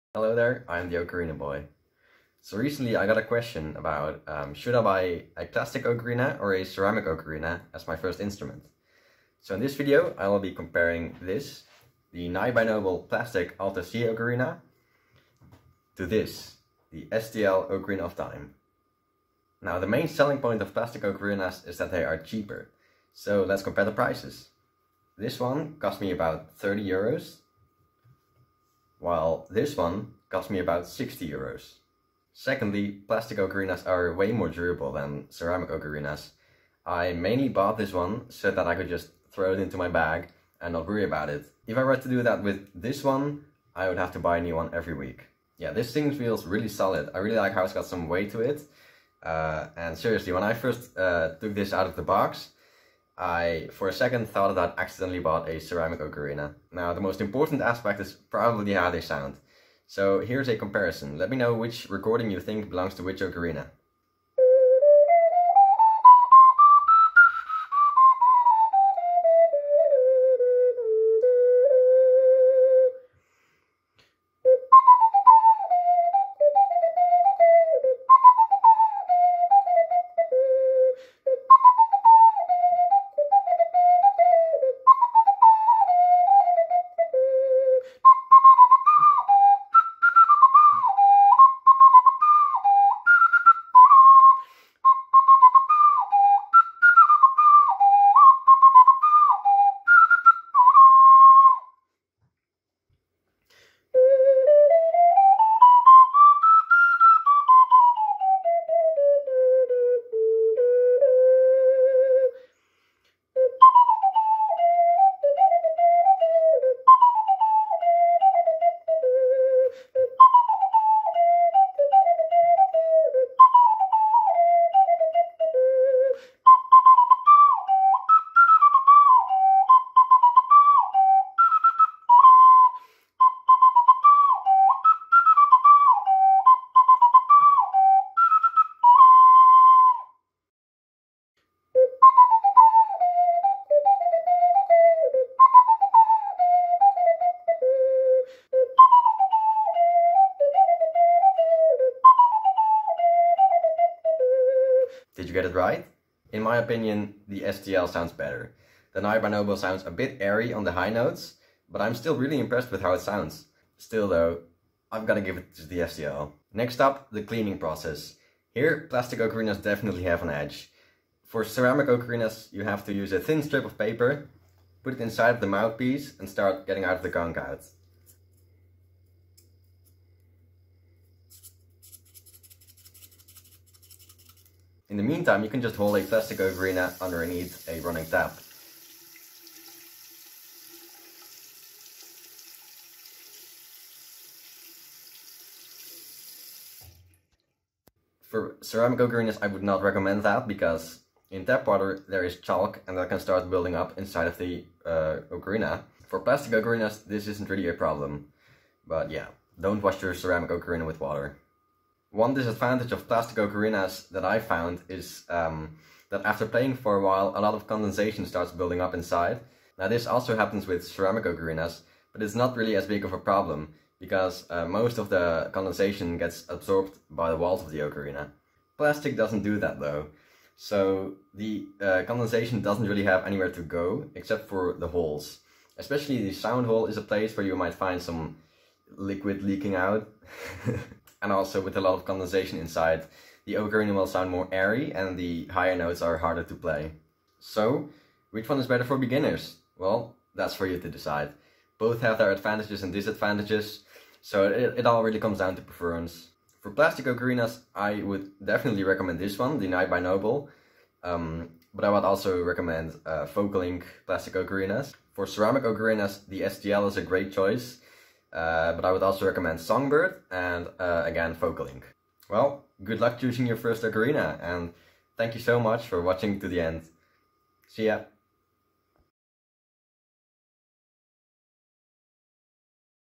Should you buy a plastic or a ceramic ocarina?
Hi, I'm the Ocarina Boy! I play the ocarina, an ancient wind instrument.
In this video, I will go over the differences, advantages and disadvantages of plastic and ceramic ocarinas.

The ocarinas I used in this video:
- Night by Noble plastic ocarina AC
- STL 12 hole Zelda tenor ocarina

Ocarinas also recommended:
- Focalink ocarina
- Songbird ocarina